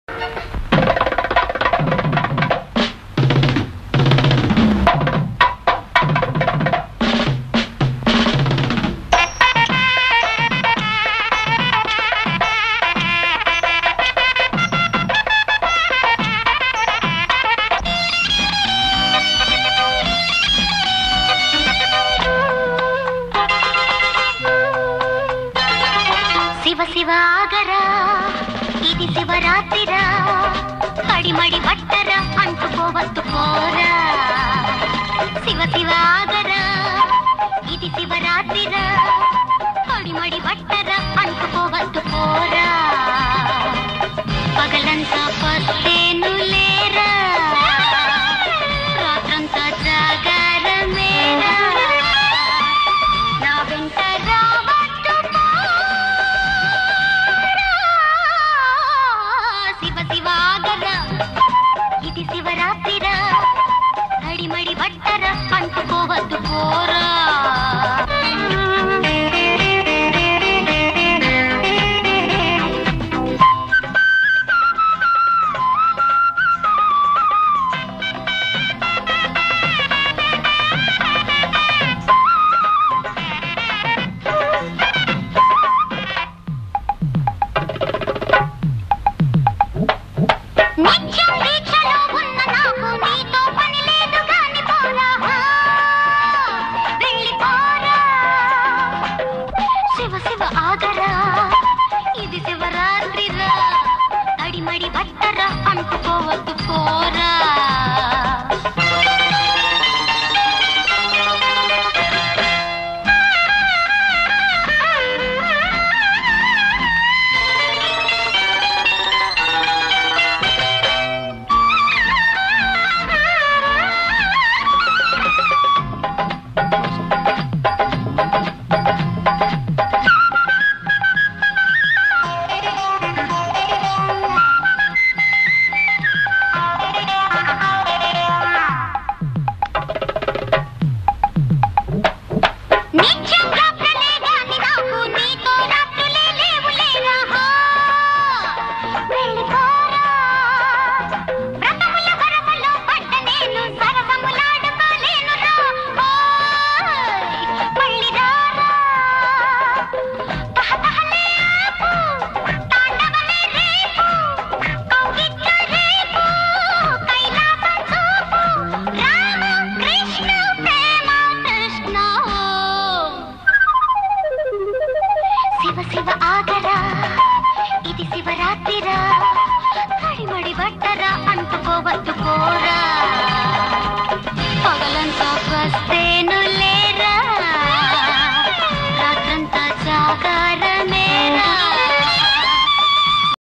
Jayamalini red hot show in wet saree
Jayamalini delights in a soaked red saree giving full view of her delicious boobs and fat round ass.